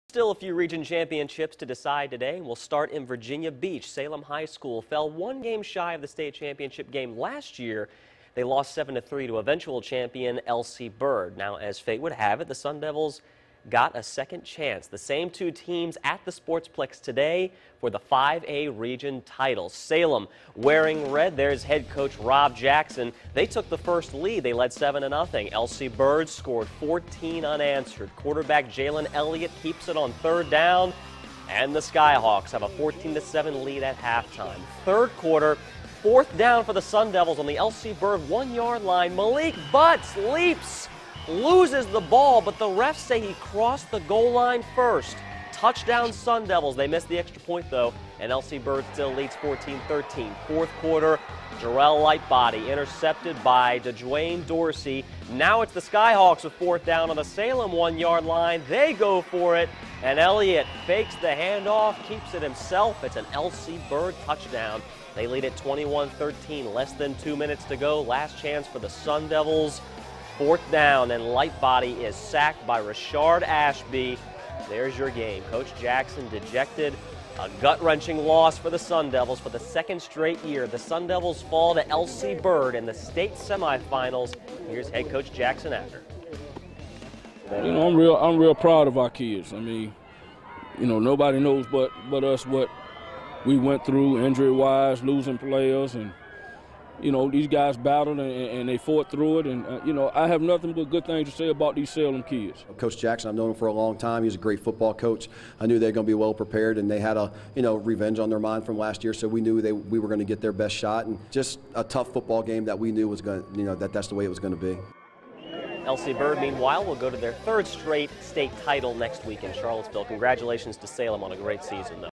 Salem falls short of state title game with 21-13 loss to LC Bird
For the second straight season, the Salem Sun Devils' season comes to an end in the Region Championship game, at the hands of LC Bird. The Sun Devils fought to the end, but fell just short with a 21-13 loss.